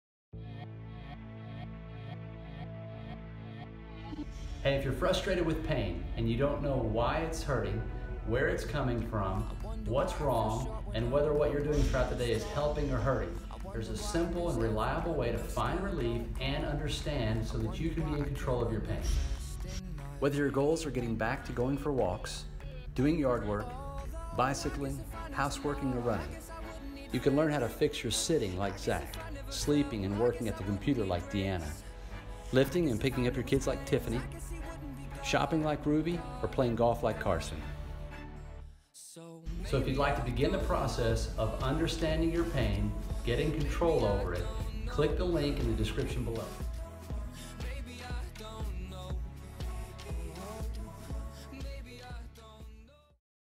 Don't Know Why You're Hurting? Jason Ward PT
Begin learning how to:
-  relieve your pain,
- recover your ability to do what you love,
- know that you're not doing more harm by continuing and
- prevent future recurrences!